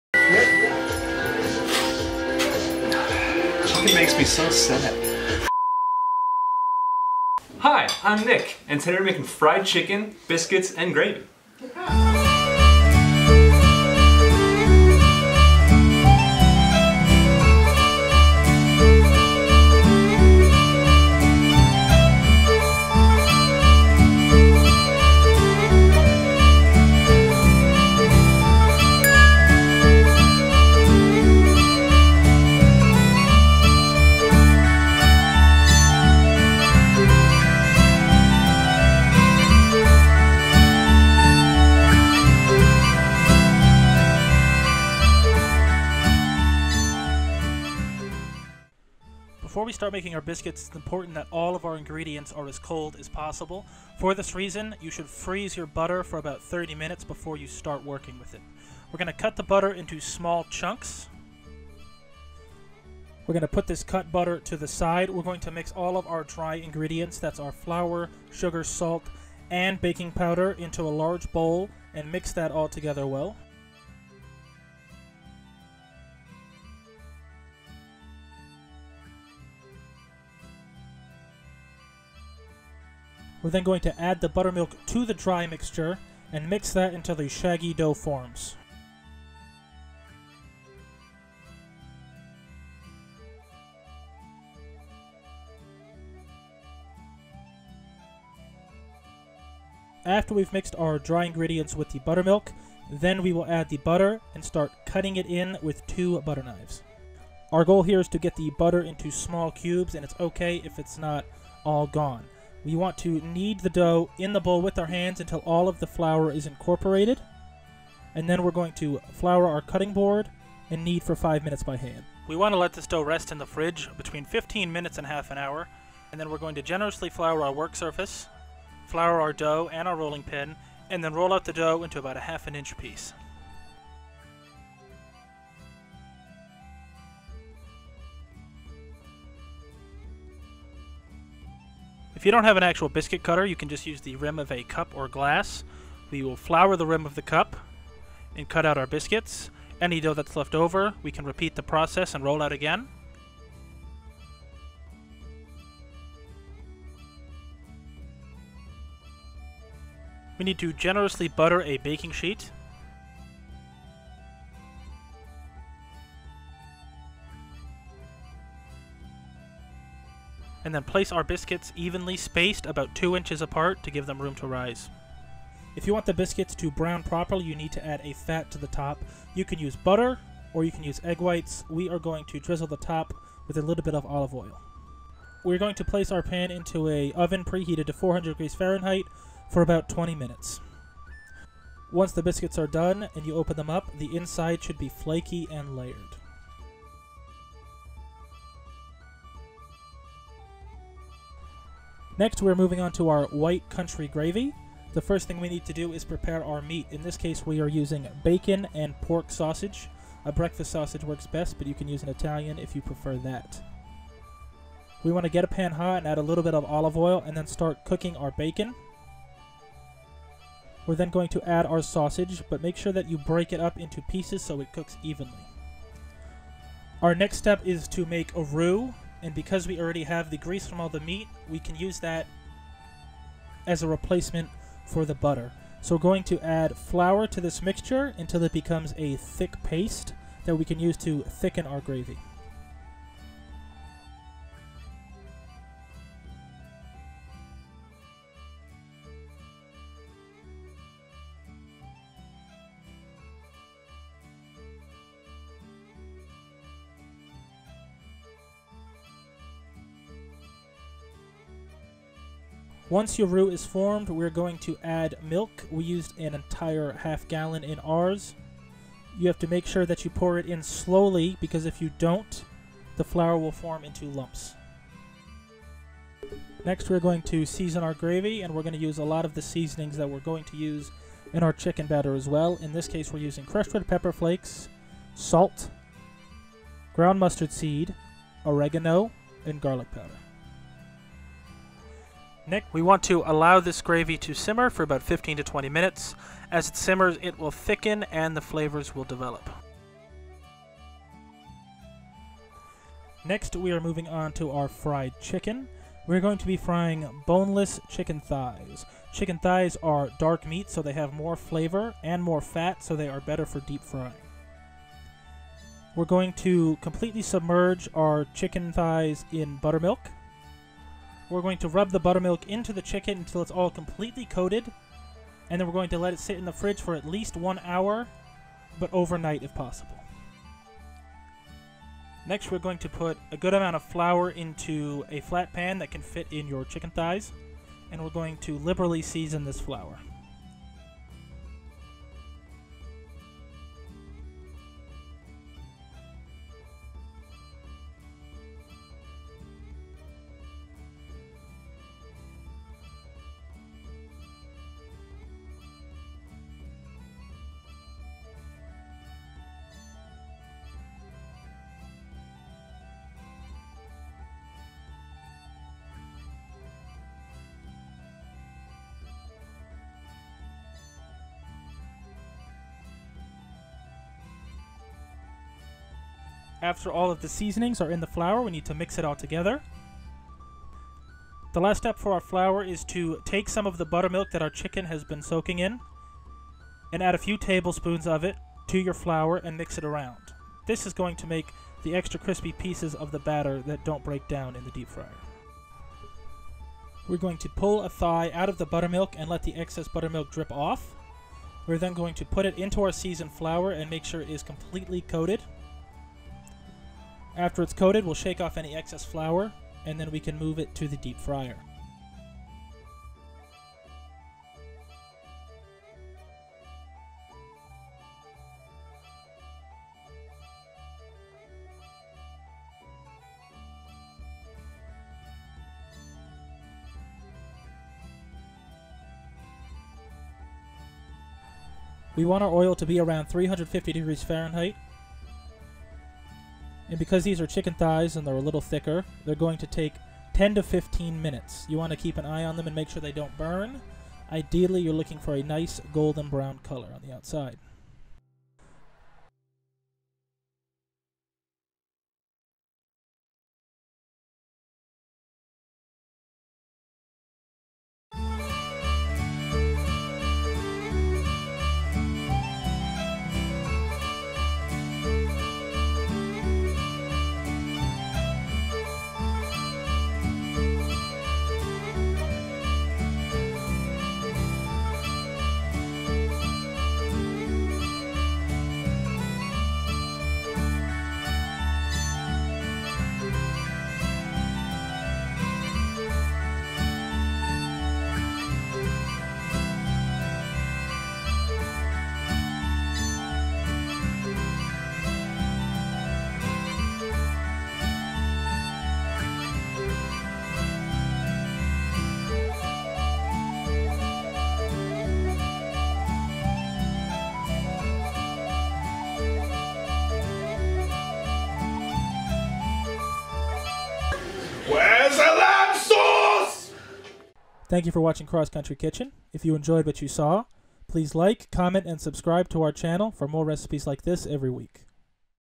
How to Make Chicken and Biscuits- Cross Country Kitchen Ep. 3
This week on Cross Country Kitchen we take you to Georgia for Chicken and Biscuits!!

We will be showing you how to make homemade buttermilk biscuits, white country gravy, and fried chicken.